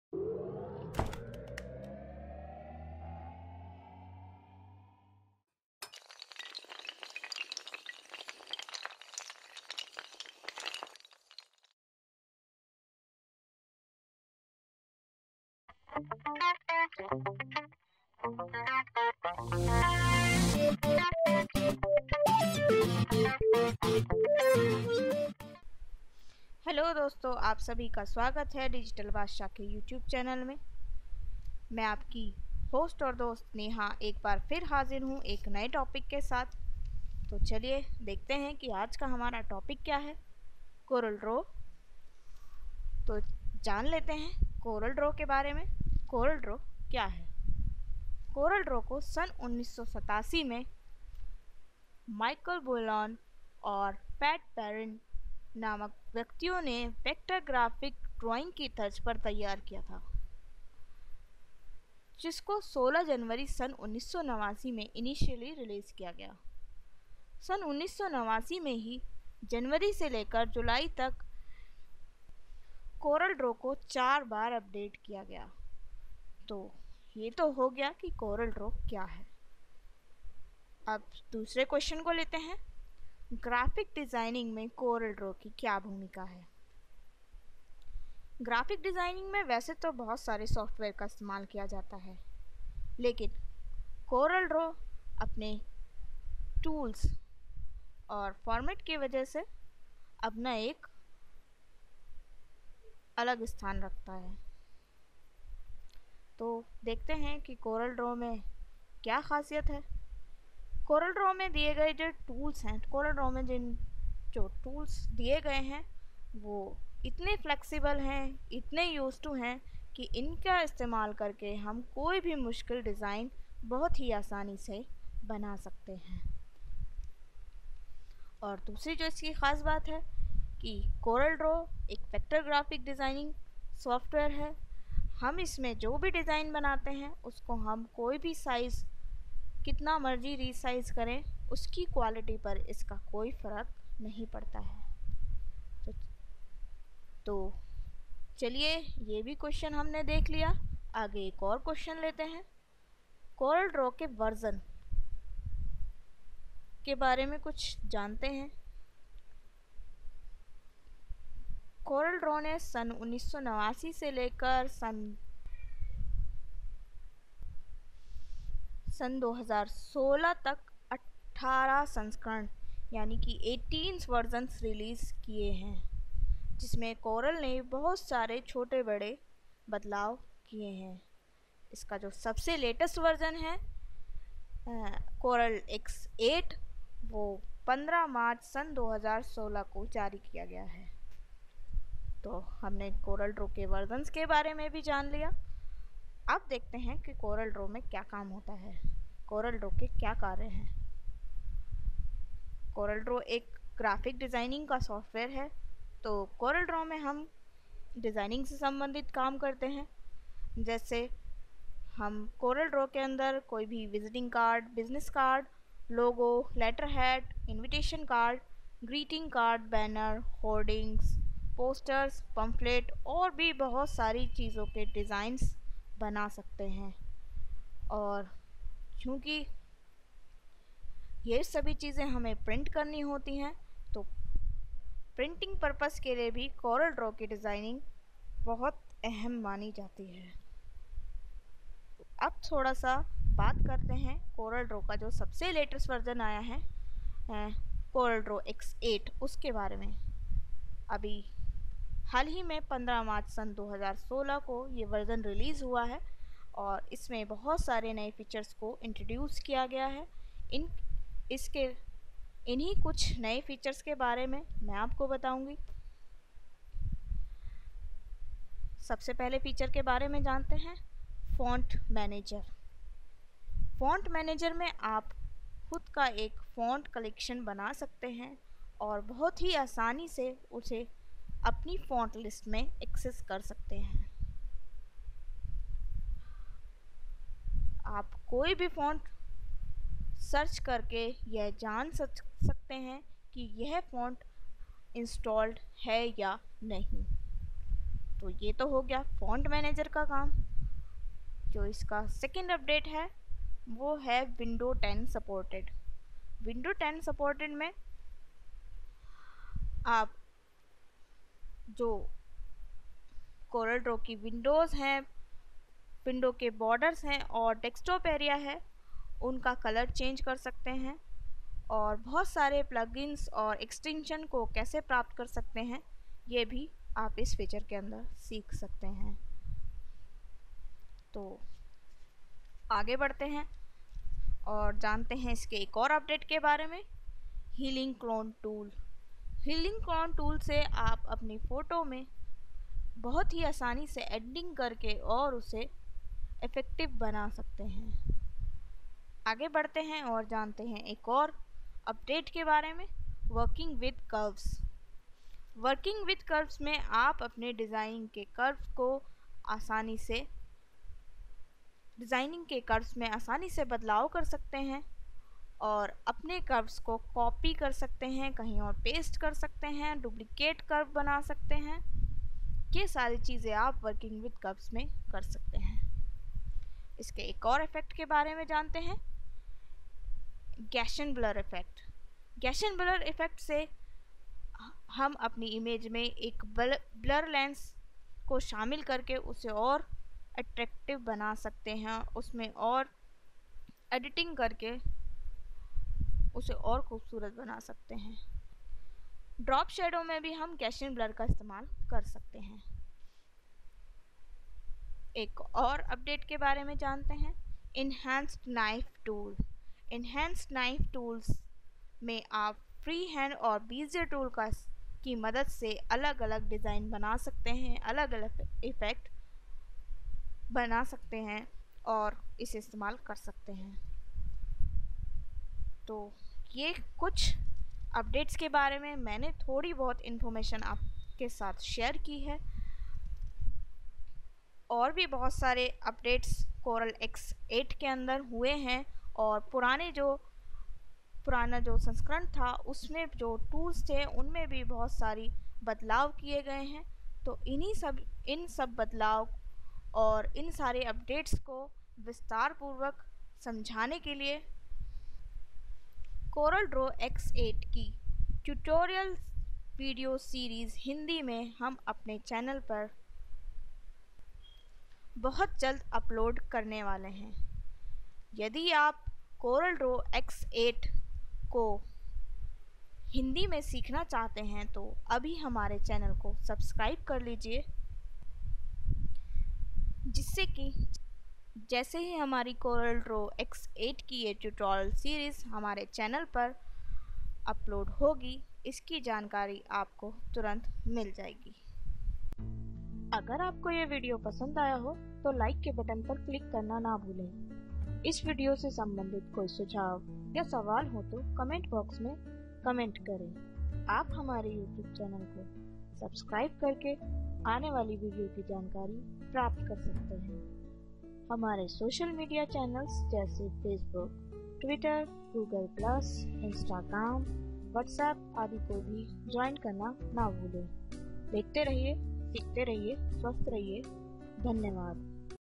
Coral Draw X8 Tutorials for beginners hindi videos - Introduction
इन Coral Draw X8 Tutorial videos के माध्यम से हमने Coral Draw X8 के सभी टूल्स और Updates को उदाहरण के साथ हिन्दी में समझाने का प्रयास किया है। जिससे आप Coral Draw X8 का उपयोग करके बहुत ही आसानी से कोई Logo, Poster, Visiting Card, Hoardings आदि बना सके।